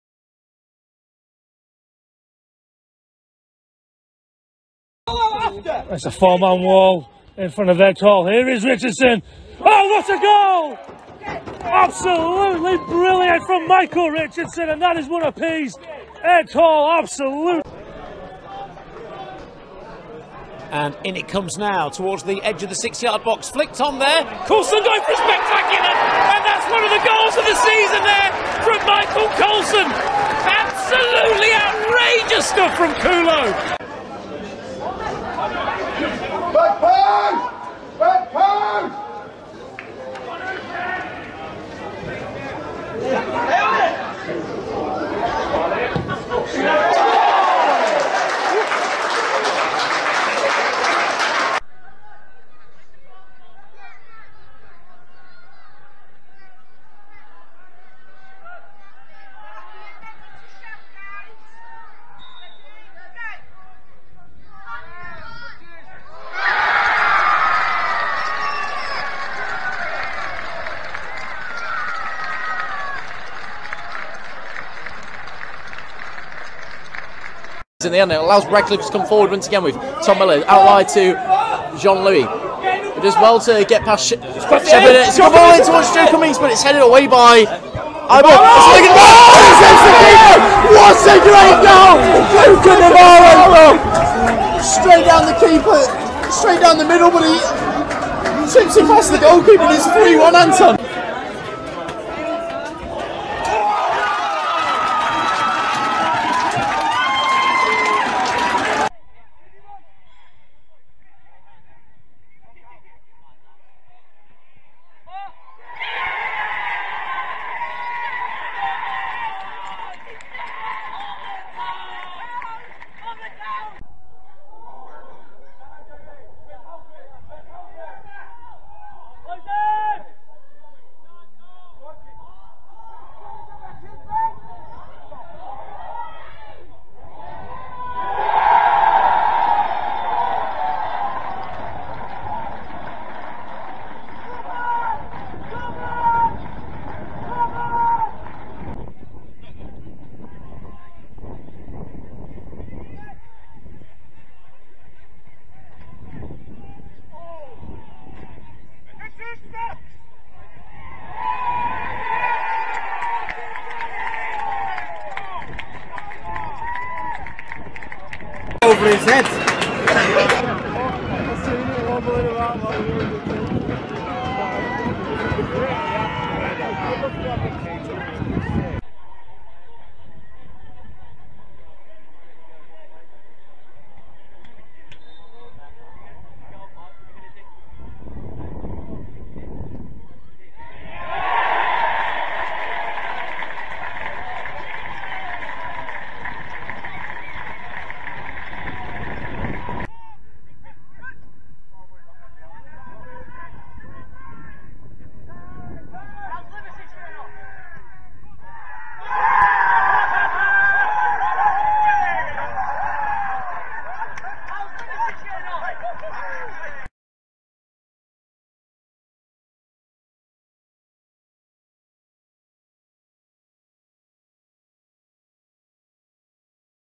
Pitching In Goal of the Month - April
The season may be over, but we still have one final clash to settle with the April Goal of the Month contest, with the dozen league goals selected by the judges ready to roll

Voting ends Tuesday 4th May 9pm